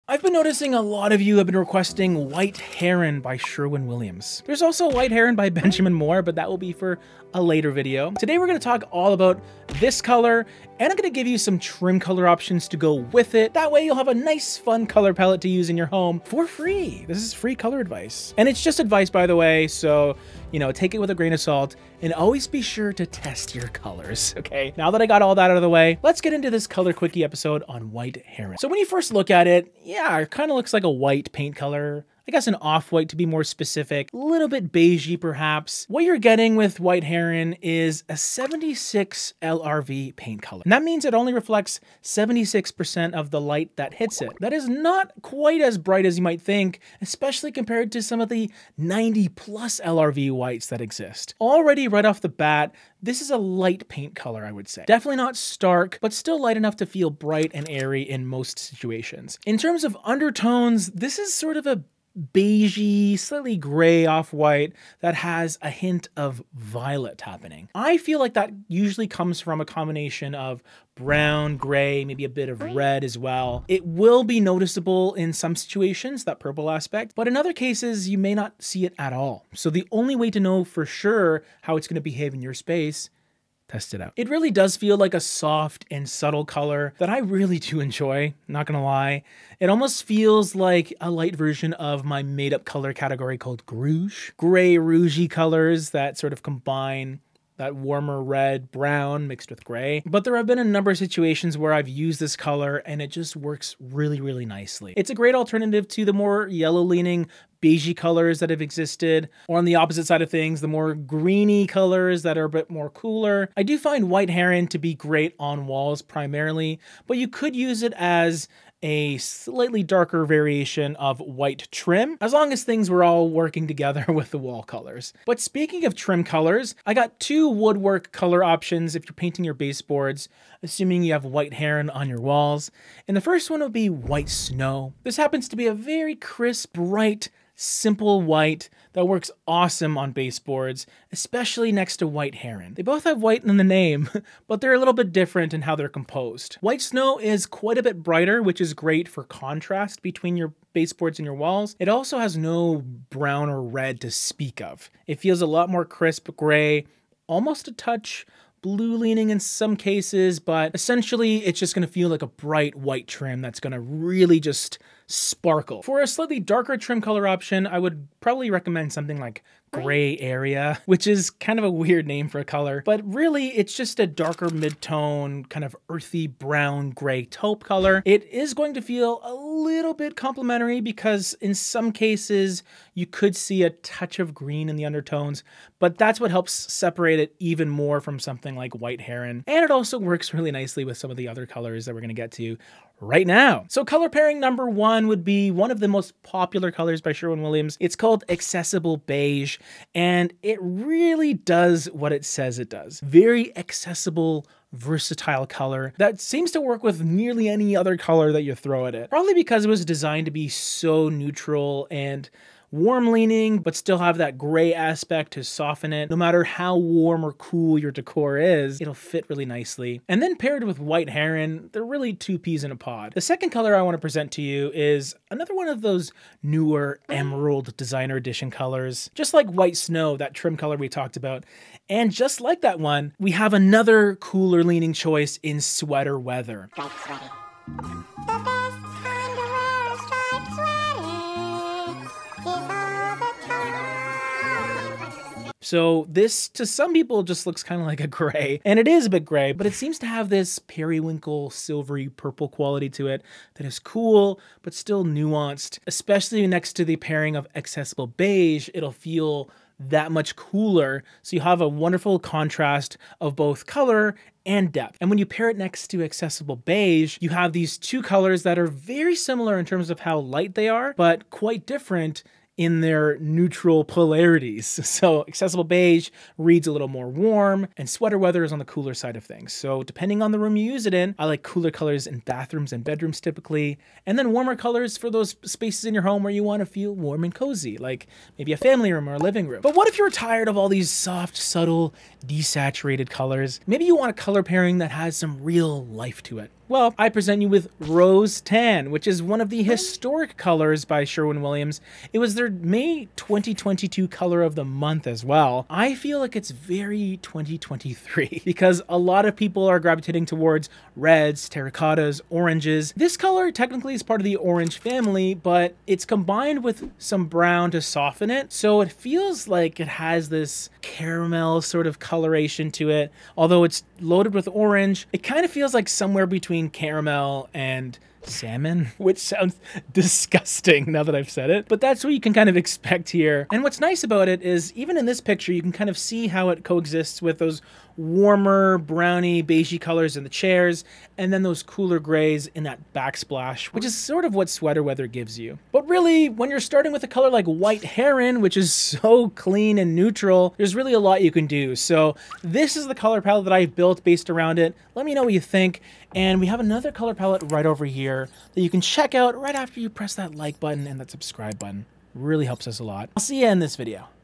Why Sherwin Williams "White Heron" is the Perfect Choice
Custom Color Palettes In this video, we're reviewing Sherwin Williams' popular "White Heron" SW 7627 paint color. We discuss what kind of color it is, some trim colors that go with it, and a color palette at the end!

If you're looking for a change of scenery and want to paint your home a beautiful white, then check out Sherwin Williams' "White Heron" paint color! We'll discuss how to best use it in your home, for your next painting project!